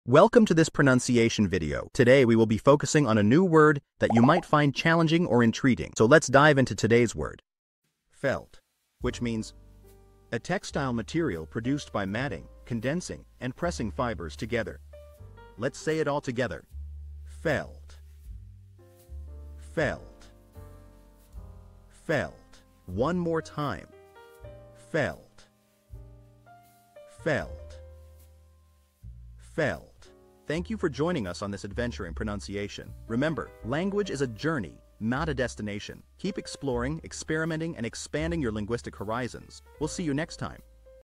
How to pronounce Felt
Master the Pronunciation of 'Felt' - which means : A textile material produced by matting, condensing, and pressing fibers together. 📝 with @PPrononciation 🎓🗣️ - [Your Guide to English]

Learn to pronounce 'Felt' perfectly in English with our easy and engaging tutorial by @PPrononciation! 📖📽️ In this comprehensive video, we provide you with native speaker examples 🗣️👂 and phonetic breakdowns 🎵 to ensure you get the pronunciation just right.

How to say Felt in English